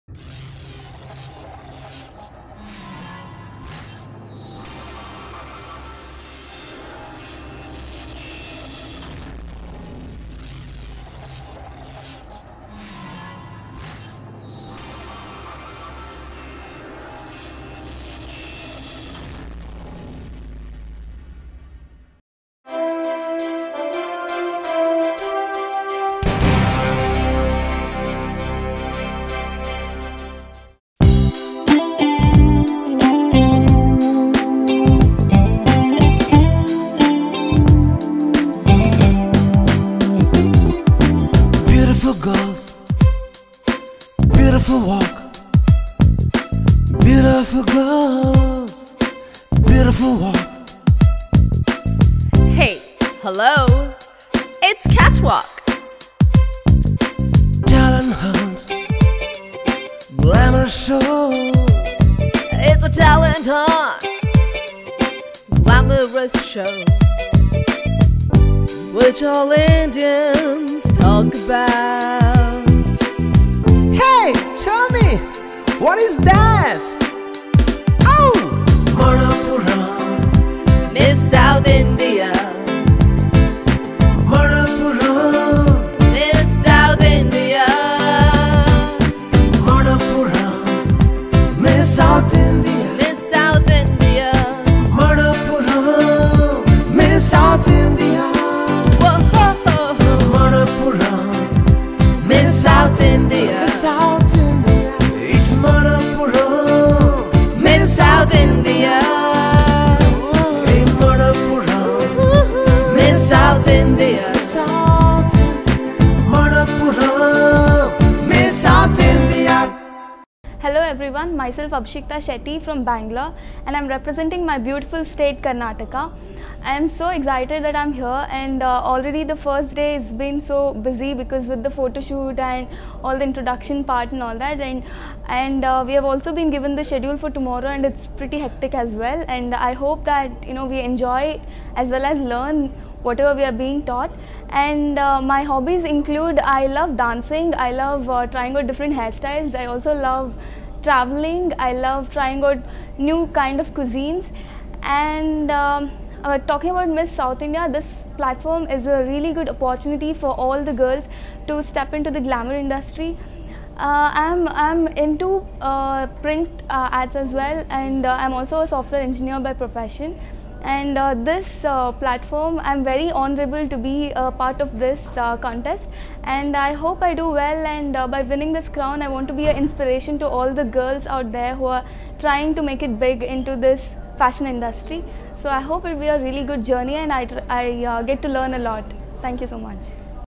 Miss South India 2014 contestant Abhishikta S Shetty
Manappuram Miss South India 2014 contestant Abhishikta S Shetty saying. A Pegasus event. An event directed by Ajit Ravi Pegasus